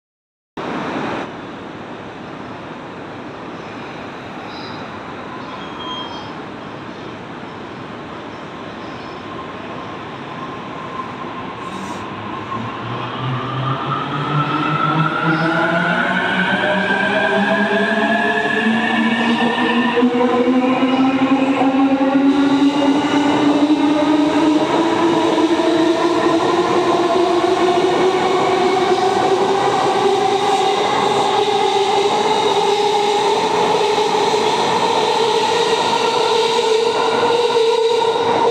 E233系3000番台・E231系1000番台コツE-16編成+コツS-23編成横浜駅発車
2859Y
湘南新宿ライン東海道線直通快速国府津行